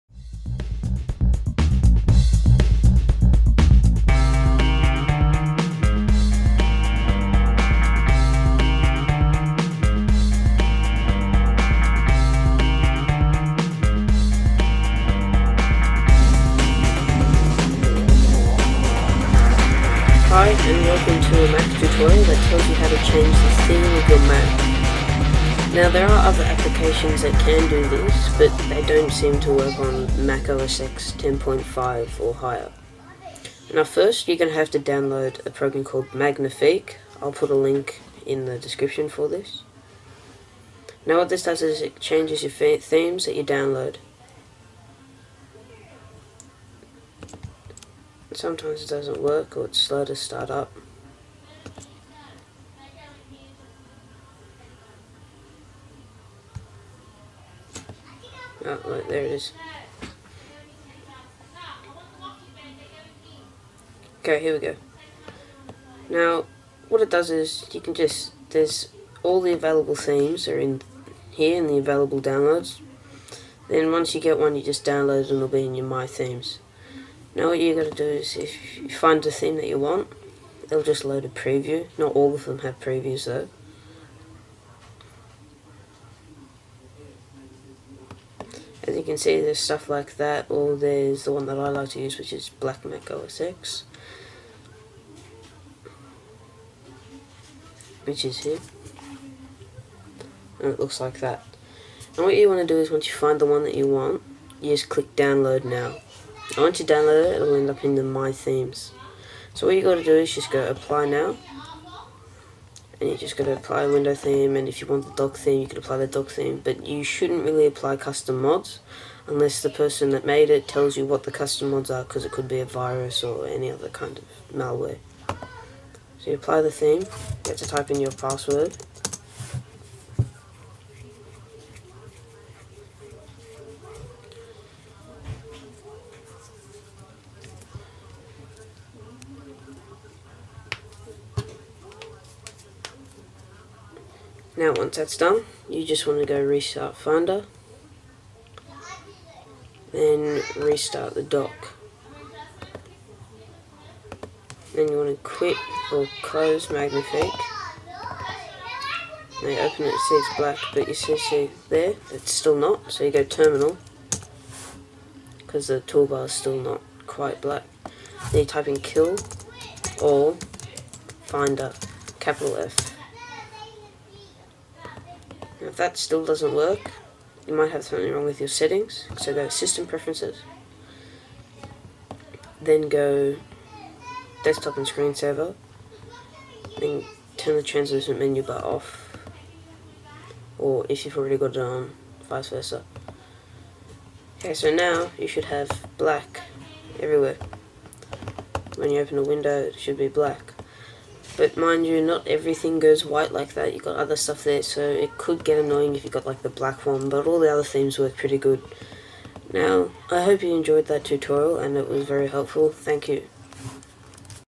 Easy Way To Change Mac Theme
This HD RIP video shows you how to hack your Mac OSX 10.5 or above theme and to add in new cool themes instead of just one theme.

☆☆☆☆☆☆Video by Tanazeftino1 and Intro and Music by Chamathi7☆☆☆☆☆☆

WARNING!!! THIS DOES NOT WORK ON MAC OSX SNOW LEOPARD AND WILL STUFF IT UP BADLY!